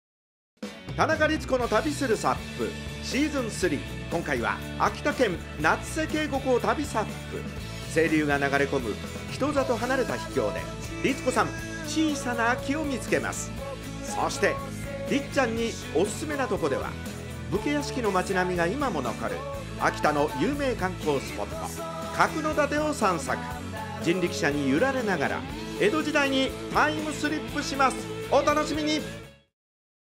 2019年11月10日 秋田 夏瀬渓谷編
【今回のコース紹介】
秋田県　夏瀬渓谷を巡ります。エメラルドブルーの水と、春は新緑の緑、秋は美しい紅葉の世界が広がります。